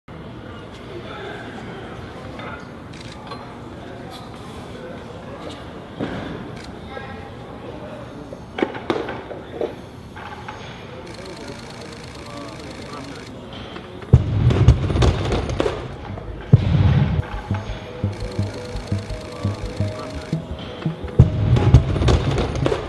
Boyanka Kostova (58kg) 180kg Front Squat 2015 World Weightlifting Championships
Boyanka Kostova (58kg Azerbaijan) 180kg Front Squat 2015 World Weightlifting Championships